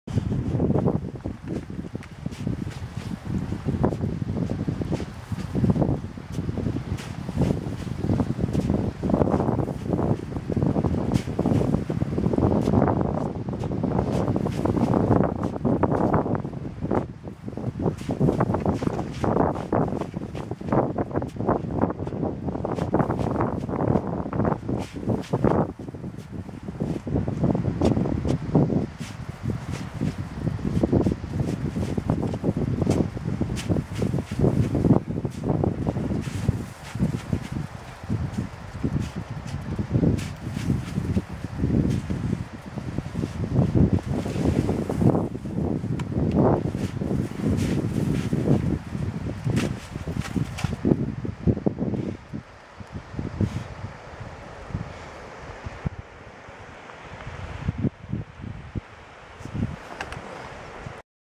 Snow is not always your friend
Snow is not your friend when slopes are more then 45 degrees and dropping about 500 meters straight down into the valley below ;)